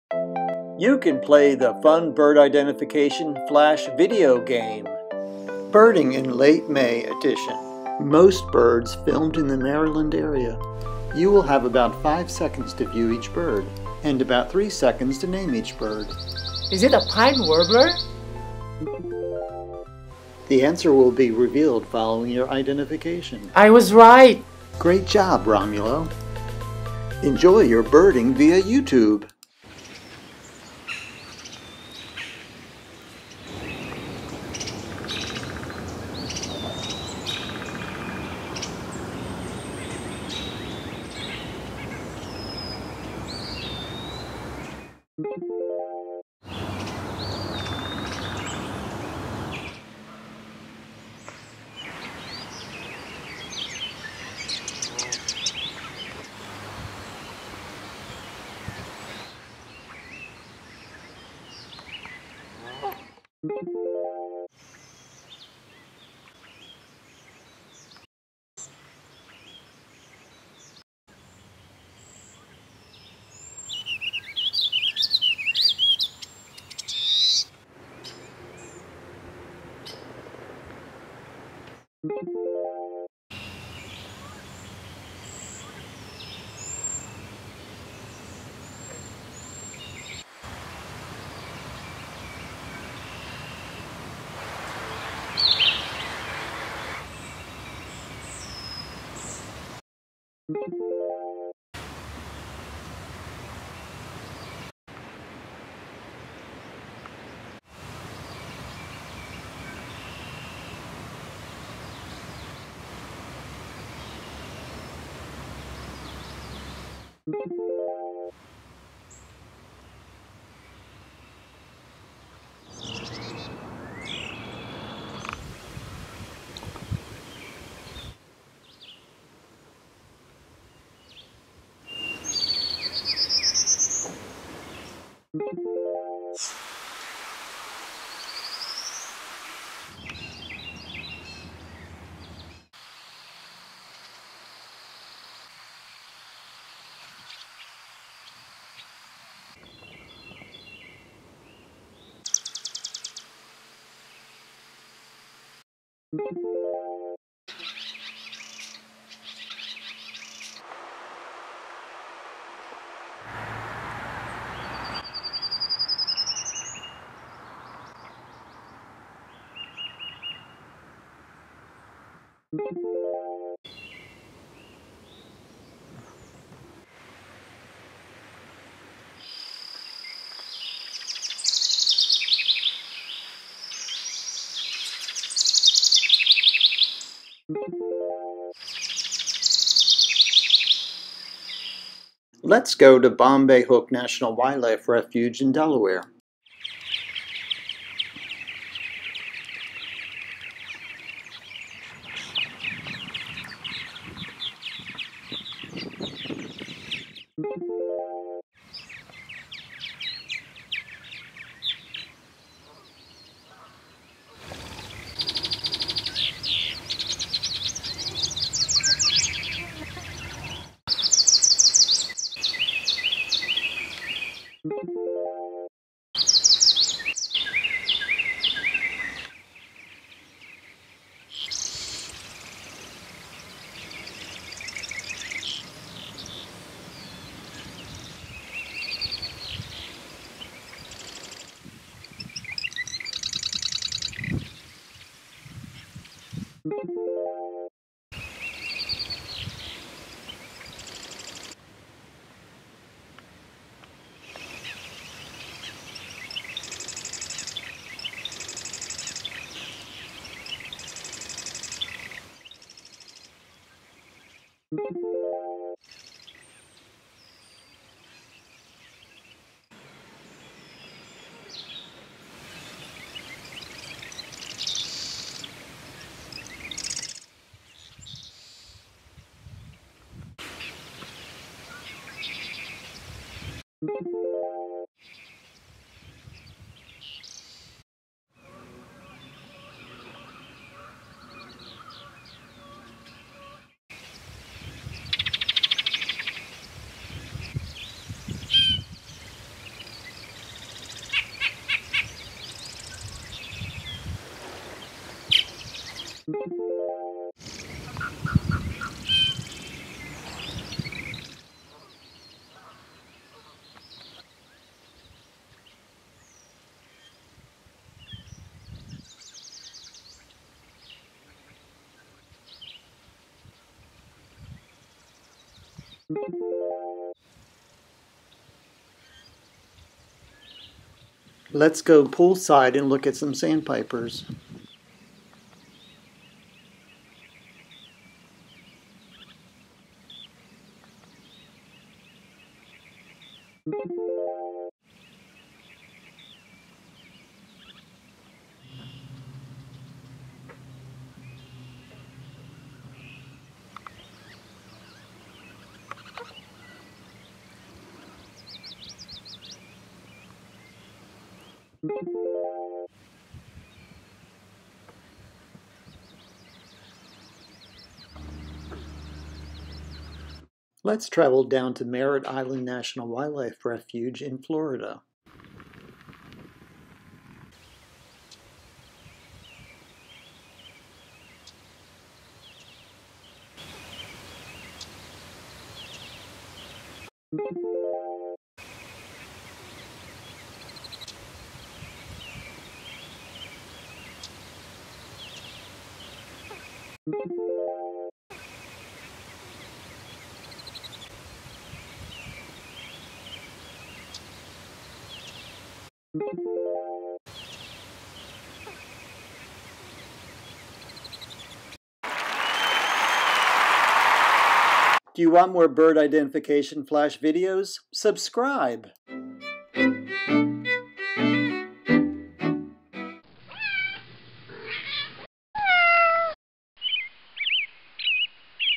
22 Fantastic Late May Birds, Fun Flash Video Quiz Game
Learn 22 Eastern United States birds with this fun video flash card game. Birds filmed in live action in late May before summer. Play the game several times to become more familiar with birds around you. View each birds for about 8 seconds and then you are prompted to name the bird. Immediate feedback on your identification!  Special visits to Bombay Hook National Wildlife Refuge and Merritt Island National Wildlife Refuge.